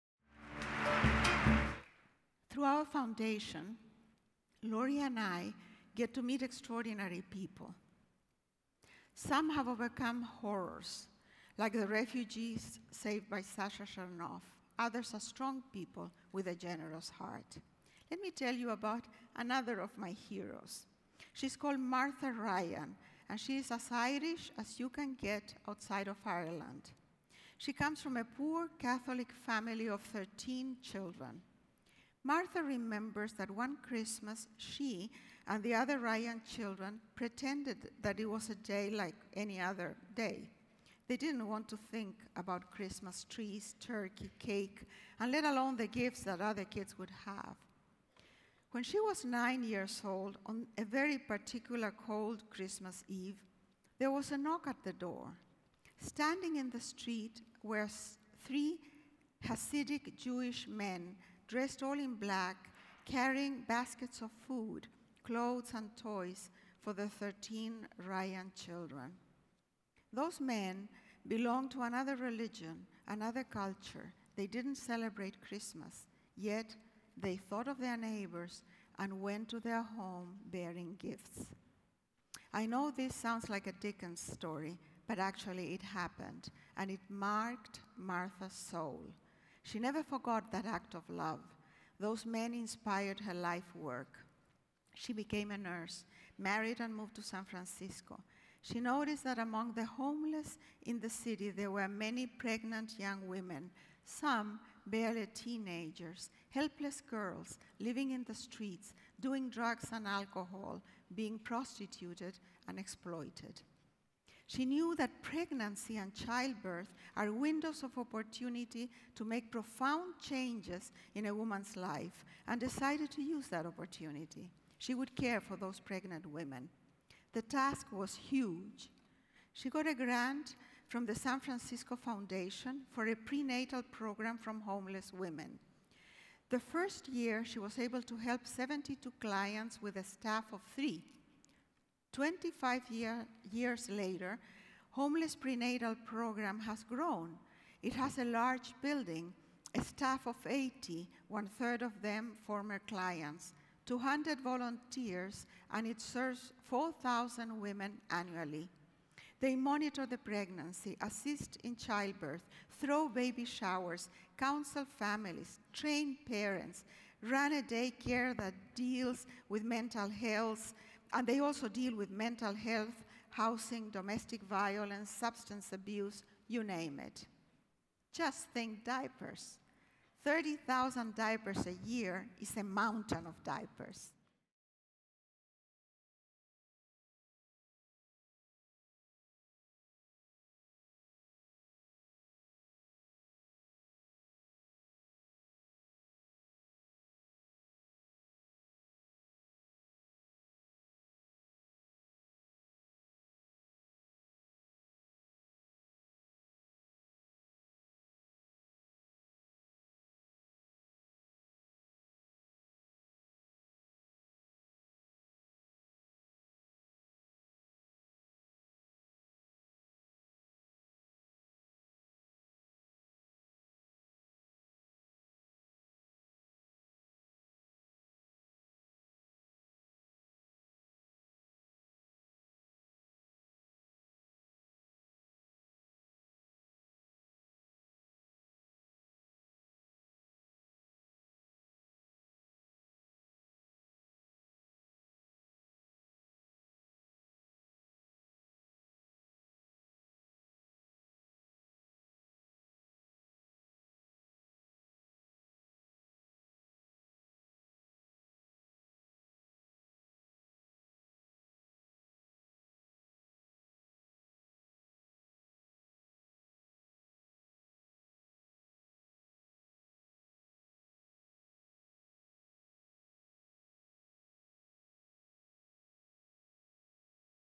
IsabelAllendeAFP Martha Ryan & HPP
Isabel Allende at the Association of Fundraising Professionals speaking about Martha Ryan and Homeless Prenatal